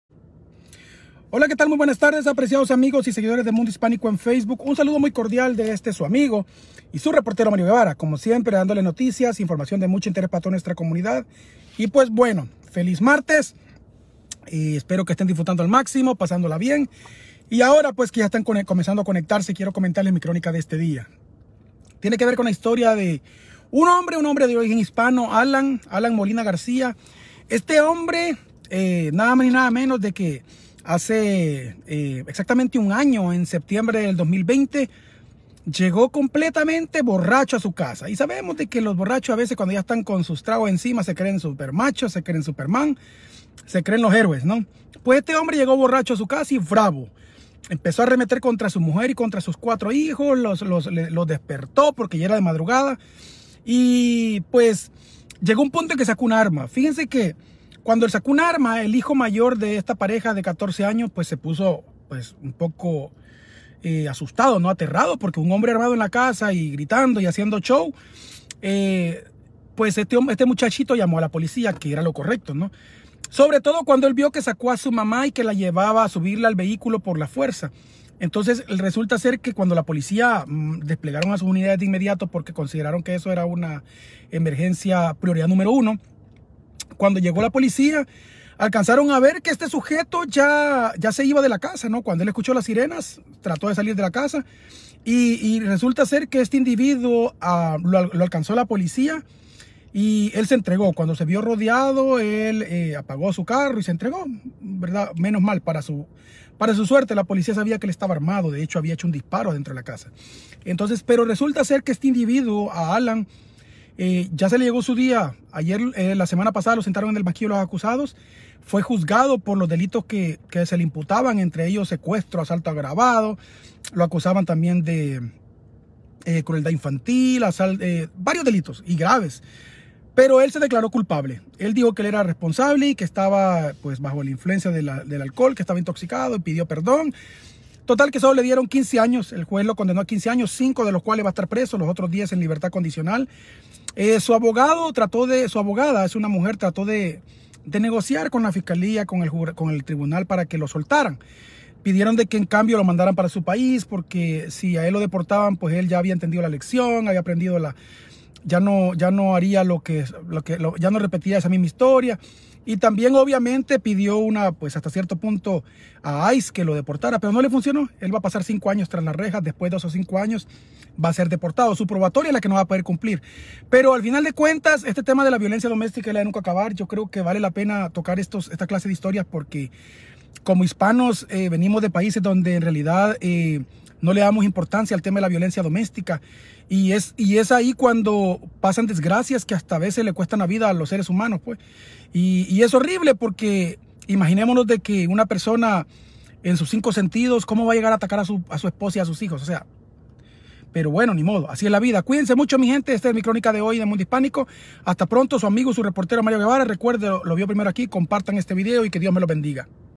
Hispano enfrentaría hasta la deportación tras desatar ola de violencia Noticias
No estaba en sus cabales y más tarde lo constatarían las autoridades. Un hombre identificado como Luis Enrique Fernández, de 30 años, fue arrestado luego de golpear a su novia, sacarla del auto y escapar con sus dos niños adentro hasta originar una persecución policial por varias ciudades de Arizona.

Fox 10 reportó el domingo 31 de octubre que Luis Fernández, un sospechoso de violencia doméstica, ya está en custodia después de que, según la policía, ocasionó una persecución imprudente a través de la zona de Phoenix el sábado por la noche mientras llevaba a dos niños pequeños dentro de un auto.